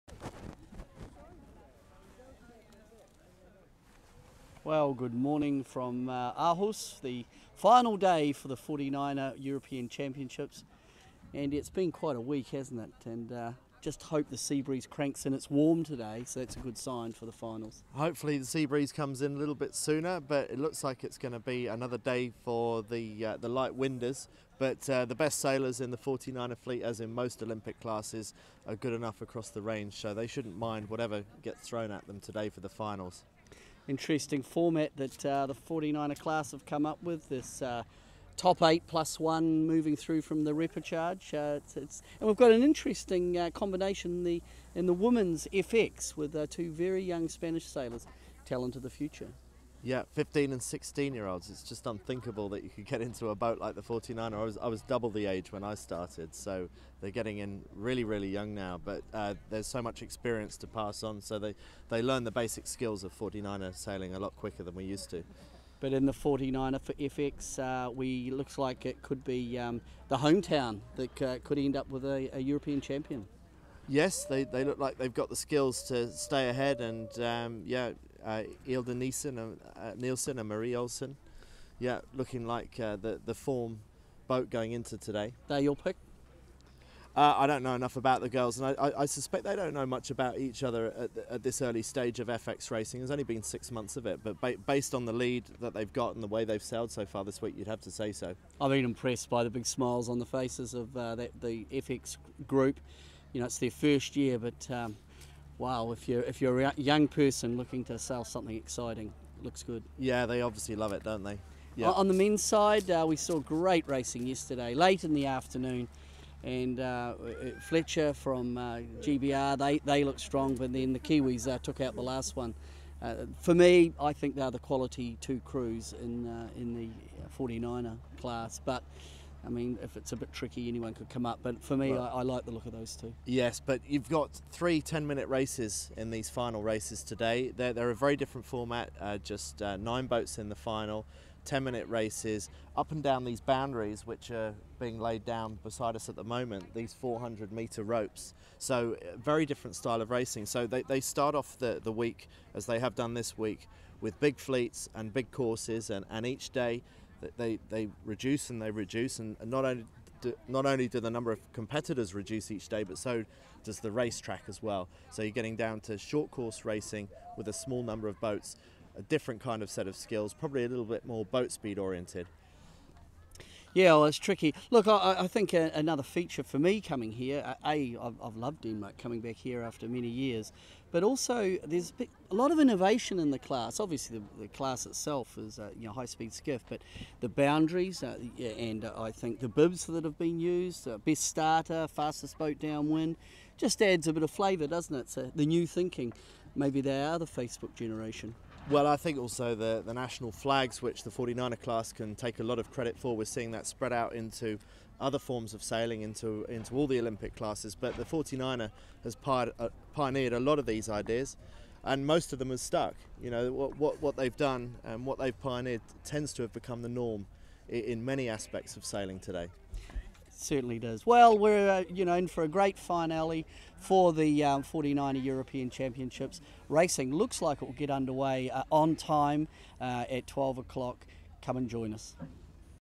2013 SAP 49er Europeans - Day 6 - Morning Show
Peter Lester and Andy Rice talk through what's happening in the morning before the finals for the 2013 49er & 49erFX European Championship.